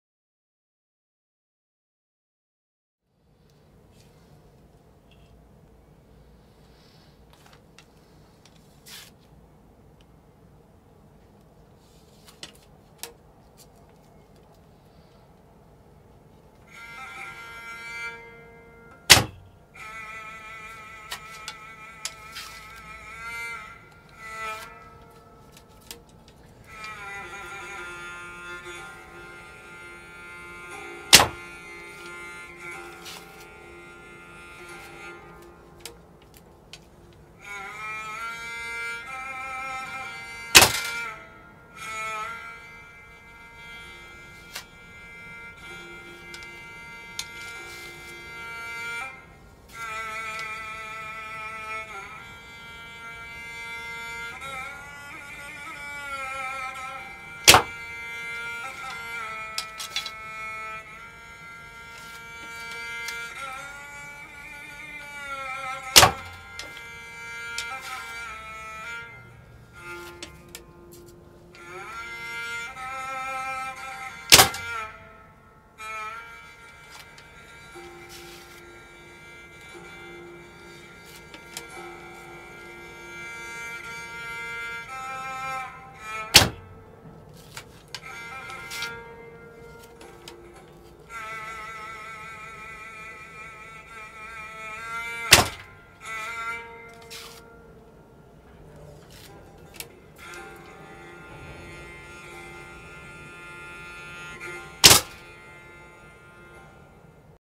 Ambidextrous Archery: Gaozhen Practice
#🏹ambidextrouschallenge

Gaozhen training using my non dominant side - working on form at close range before trying from further away. This was very challenging at first (I have cuts & bruises to show for it) but after a few days of practice it starts to feel more natural.

Kheshig bow 45# @ 31”